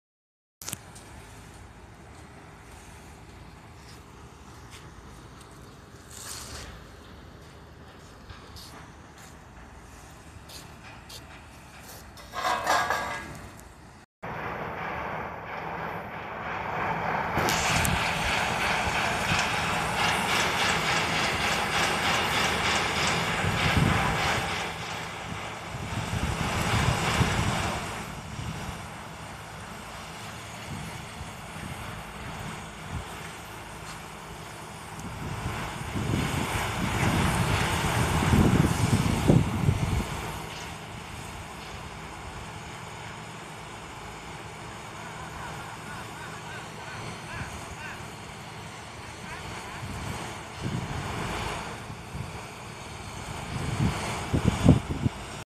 banyuwangi
banyuwangi miris sekali. putri sritanjung patah jadi dua. aset legenda kebanggaan daerah